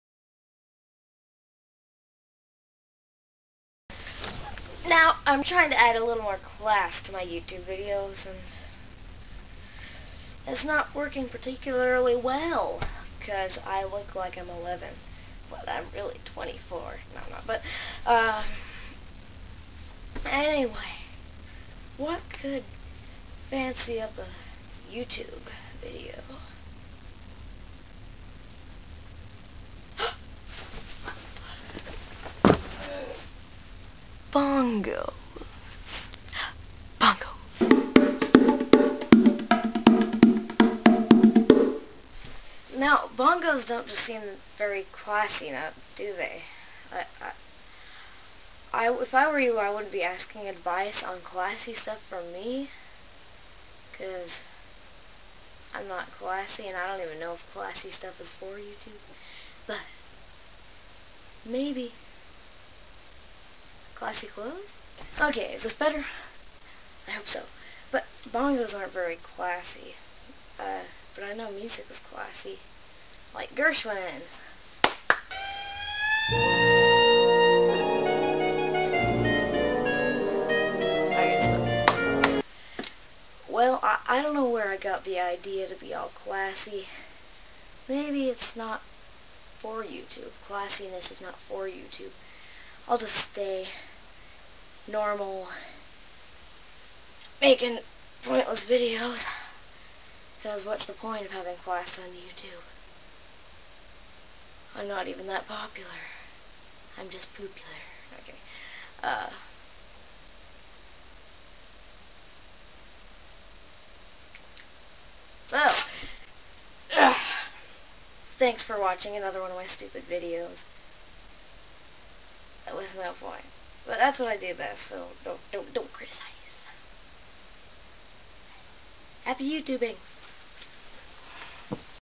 Should Youtube Be Classy?
They just aren't. Trust me. After making this video, I should know.

Here is what I said in this video, in case you for some reason wanted to know. Maybe, for example, you thought I said something and you just wanted to check if I really did. Or maybe you want to do something else with it. I don't really care. Here is what I said, split in paragraphs based on each scene I taped:

Scene 1: Now, I'm trying to add a little more class to my Youtube videos. And it's not working particularly well because I look like I'm eleven, but I'm really twenty-four. No I'm not, but anyway, what could fancy up a Youtube video? Aha! Bongos? Yeah! Bongos!

Scene 3: Now, bongos don't just seem very classy, now, do they? If I were you, I wouldn't be asking advice on classy stuff from me because I'm not classy and I don't even know if classy stuff is for Youtube. But maybe classy clothes?

Scene 4: Okay, is this better? I hope so. But the bongos aren't very classy, but I know music is classy like Gershwin! Okay, stop. Well, I don't know where I got the idea to be all classy, maybe it's not for Youtube. Classiness is not for Youtube. I'll just stay normal, making pointless videos. Because what's the point of having class on Youtube? I'm not even that popular. I'm just popular. So!

But that's what I do best, so don't criticize. Happy Youtubing!